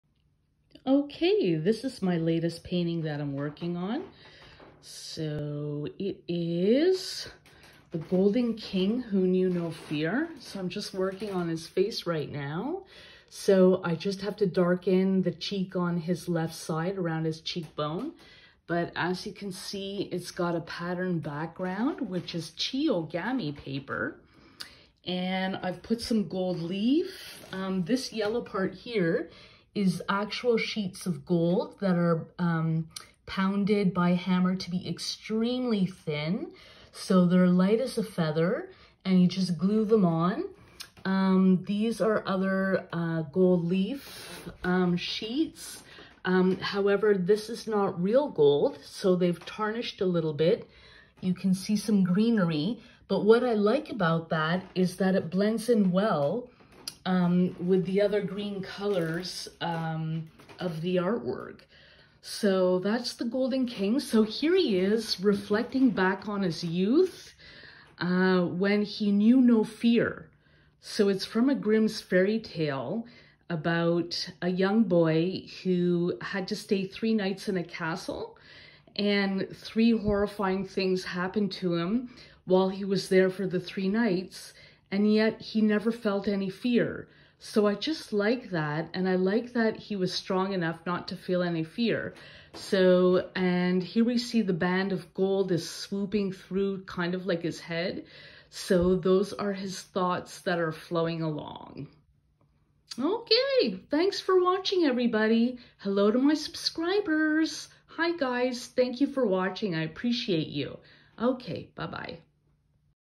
The Golden King Who Knew No Fear
This painting is called The Golden King Who Knew No Fear. It’s a mixed media work on Masonite. The painting is based on the Grimm Brothers’ fairy tale : The Youth who knew no fear.